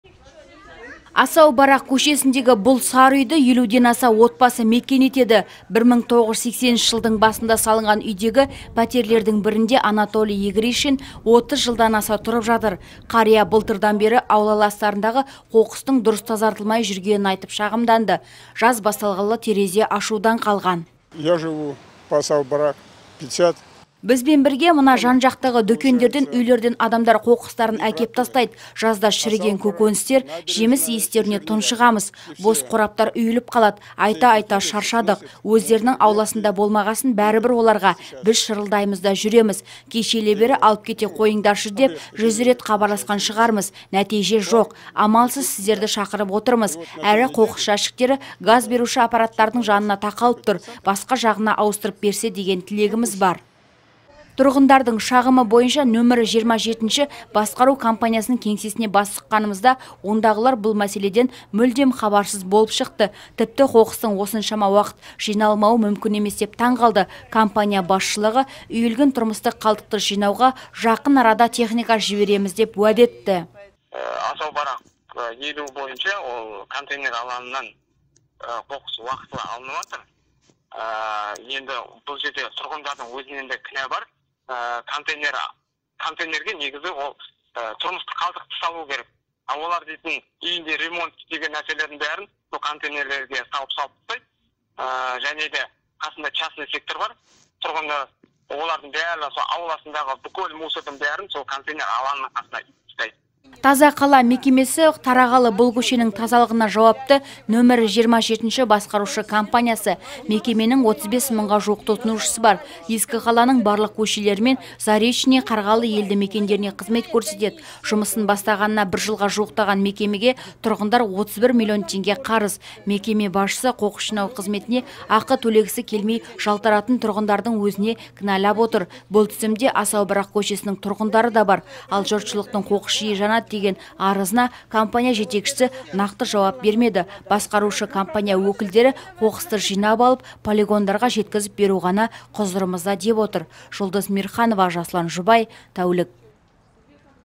Асау-Барақ көшесі қоқысқа толды
Асау-Барақ көшесіндегі №50-ші үйдің тұрғындары 3 аптадан бері қоқыс жинайтын техниканы күтіп отыр. Қоқыс жәшіктеріне сыймай, тұрмыстық қалдықтар тасып-төгіліп жатыр. Иісі ауланы алып кеткен. Тұрғындардың шағымы бойынша №27-ші басқару компаниясының кеңсесіне  бас сұққанымызда ондағылар бұл мәселеден мүлдем хабарсыз болып шықты. Тіпті, қоқыстың осыншама уақыт жиналмауы мүмкін емес деп таң қалды. Компания басшысы  үйілген тұрмыстық қалдықты жинауға жақын арада техника жібереміз деп уәде етті. "Таза қала" мекемесі тарағалы бұл көшенің тазалығына жауапты №27-ші басқару компаниясы. Мекеменің 35 мыңға жуық тұтынушысы бар. Ескі қаланың барлық көшелері мен Заречный, Қарғалы елді мекендеріне  қызмет көрсетеді. Жұмысын бастағанына  бір жылға жуықтаған мекемеге тұрғындар 31 млн теңге қарыз. Мекеме басшысы қоқыс жинау қызметіне ақы төлегісі келмей жалтаратын  тұрғындардың өзіне кінәлап отыр. Бұл тізімде Асау-Барақ көшесінің тұрғындары да бар. Ал, жұртшылықтың қоқыс жиі жанады деген арызына компания жетекшісі  нақты жауап бермеді. Басқарушы компания өкілдері қоқысты жинап алып, полигондарға жеткізіп беру ғана құзырымызда деп отыр.